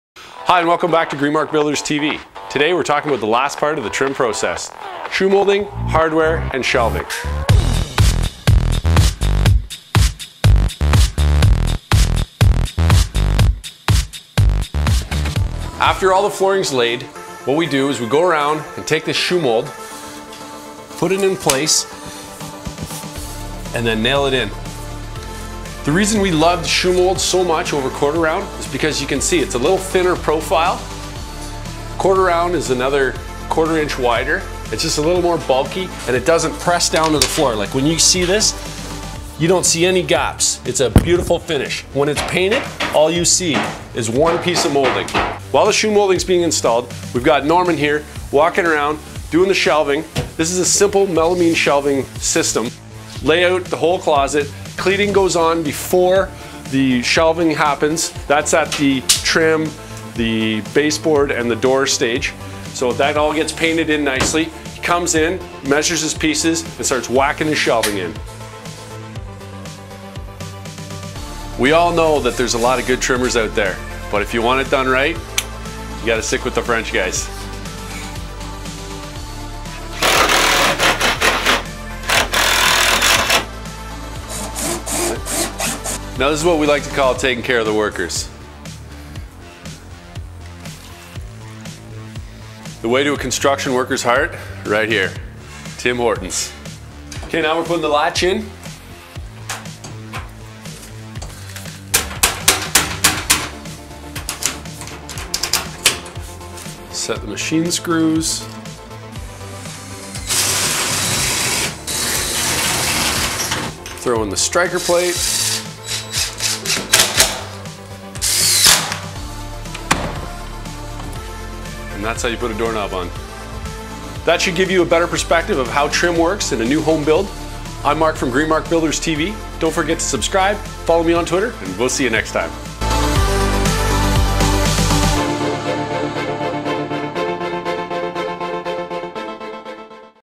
Trimming Closets, Shoe Moulding and Knobs
The 2nd part of the trim process on a new build is pulling all the loose ends together.  Mark Kranenburg from Greenmark Builders TV shows you the importance of shoe moulding,how to install closet shelves, door knobs and strikers.